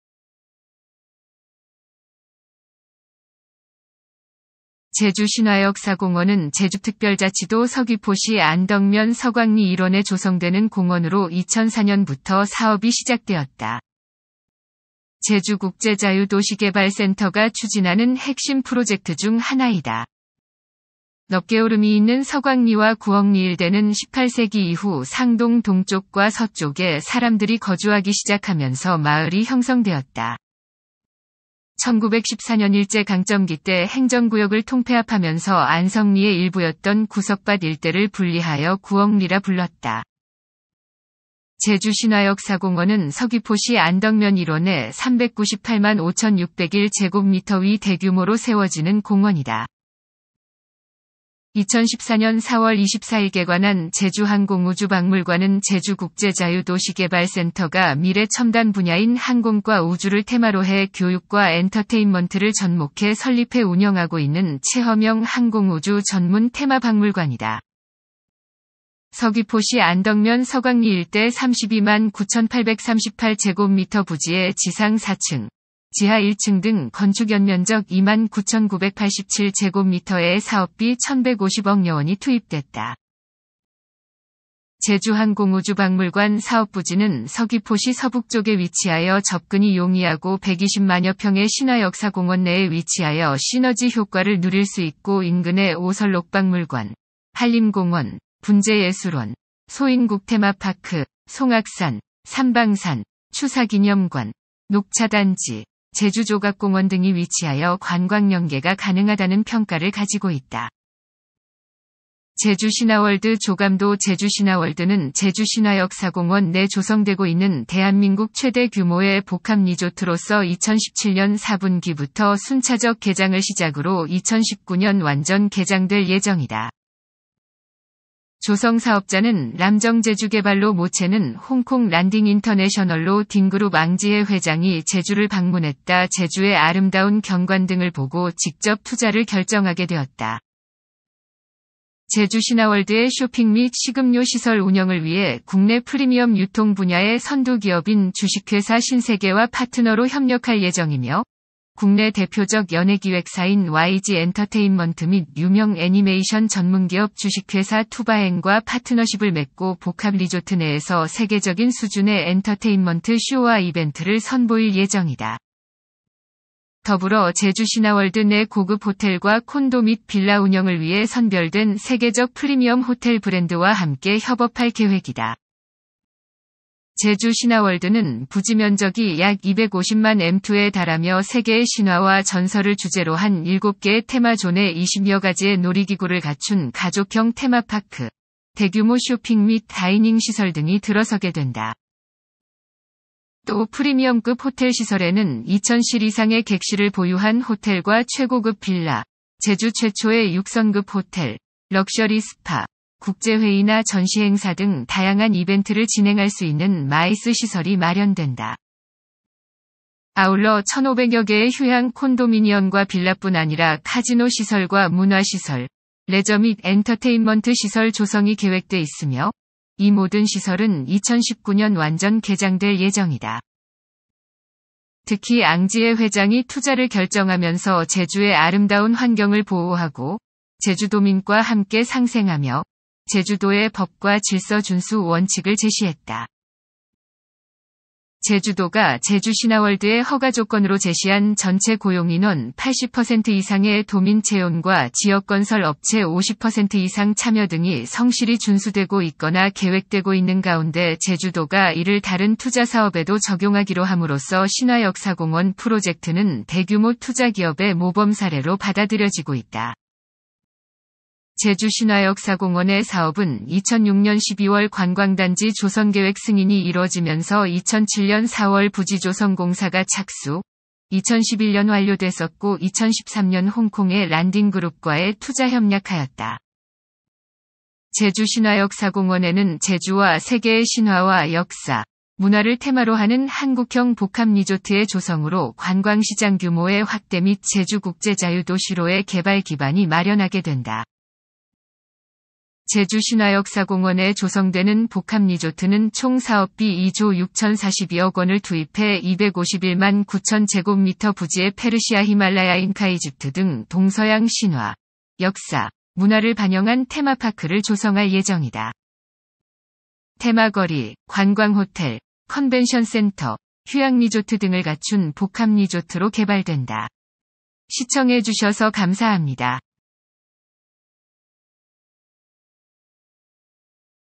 제주신화역사공원
제주신화역사공원(濟州神話歷史公園, Jeju Myths and History Theme Park)은 제주특별자치도 서귀포시 안덕면 서광리 일원에 조성되는 공원으로 2004년부터 사업이 시작되었다.
 제주국제자유도시개발센터(JDC)가 추진하는 핵심 프로젝트 중 하나이다.
 넙게오름이 있는 서광리와 구억리일대는 18세기 이후 상동 동쪽과 서쪽에 사람들이 거주하기 시작하면서 마을이 형성되었다.
 1914년 일제 강점기 때 행정구역을 통폐합하면서 안성리의 일부였던 ‘구석밧’ 일대를 분리하여 구억리라 불렀다.
 제주신화역사공원은 서귀포시 안덕면 일원에 3,985,601m2(약 121만평)의 대규모로 세워지는 공원이다.
 2014년 4월 24일 개관한 제주항공우주박물관(Jeju Aerospace Museum, JAM)은 제주국제자유도시개발센터(JDC)가 미래 첨단 분야인 항공과 우주를 테마로 해 교육과 엔터테인먼트를 접목해 설립해 운영하고 있는 체험형 항공우주 전문 테마박물관이다.
 서귀포시 안덕면 서광리 일대 32만9838m2(약 10만평) 부지에 지상 4층(전망대 포함), 지하 1층 등 건축 연면적 2만9987m2(약 9100평)에 사업비 1150억여원이 투입됐다.
 제주항공우주박물관 사업부지는 서귀포시 서북쪽에 위치하여 접근이 용이하고 120만여평의 신화역사공원내에 위치하여 시너지 효과를 누릴 수 있고 인근에 오설록박물관, 한림공원, 분재예술원, 소인국테마파크, 송악산, 산방산, 추사기념관, 녹차단지, 제주조각공원 등이 위치하여 관광연계가 가능하다는 평가를 가지고 있다.
 제주신화월드 조감도 제주신화월드는 제주신화역사공원 내 조성되고 있는 대한민국 최대 규모의 복합리조트로서 2017년 4분기부터 순차적 개장을 시작으로 2019년 완전 개장될 예정이다.
 조성 사업자는 람정제주개발(주)로 모체는 홍콩 란딩인터내셔널로 딩그룹 앙지혜(仰智慧) 회장이 제주를 방문했다 제주의 아름다운 경관 등을 보고 직접 투자를 결정하게 되었다.
 제주신화월드의 쇼핑 및 식음료 시설 운영을 위해 국내 프리미엄 유통 분야의 선두 기업인 ㈜신세계와 파트너로 협력할 예정이며, 국내 대표적 연예기획사인 YG 엔터테인먼트 및 유명 애니메이션 전문기업 ㈜투바앤과 파트너십을 맺고 복합 리조트 내에서 세계적인 수준의 엔터테인먼트 쇼와 이벤트를 선보일 예정이다.
 더불어 제주신화월드 내 고급 호텔과 콘도 및 빌라 운영을 위해 선별된 세계적 프리미엄 호텔 브랜드와 함께 협업할 계획이다.
 제주 신화월드는 부지면적이 약 250만m2에 달하며 세계의 신화와 전설을 주제로 한 7개의 테마 존에 20여 가지의 놀이기구를 갖춘 가족형 테마파크, 대규모 쇼핑 및 다이닝 시설 등이 들어서게 된다.
 또 프리미엄급 호텔 시설에는 2000실 이상의 객실을 보유한 호텔과 최고급 빌라, 제주 최초의 6성급 호텔, 럭셔리 스파, 국제회의나 전시행사 등 다양한 이벤트를 진행할 수 있는 마이스(MICE) 시설이 마련된다.
 아울러 1500여 개의 휴양 콘도미니엄과 빌라뿐 아니라 카지노 시설과 문화시설, 레저 및 엔터테인먼트 시설 조성이 계획돼 있으며, 이 모든 시설은 2019년 완전 개장될 예정이다.
 특히 앙지혜(仰智慧) 회장이 투자를 결정하면서 제주의 아름다운 환경을 보호하고, 제주도민과 함께 상생하며, 제주도의 법과 질서 준수 원칙을 제시했다.
 제주도가 제주신화월드의 허가조건으로 제시한 전체 고용인원 80% 이상의 도민 채용과 지역건설 업체 50%이상 참여 등이 성실히 준수되고 있거나 계획되고 있는 가운데 제주도가 이를 다른 투자사업에도 적용하기로 함으로써 신화역사공원 프로젝트는 대규모 투자기업의 모범사례로 받아들여지고 있다.
 제주신화역사공원의 사업은 2006년 12월 관광단지 조성계획 승인이 이뤄지면서 2007년 4월 부지조성공사가 착수, 2011년 완료됐었고 2013년 홍콩...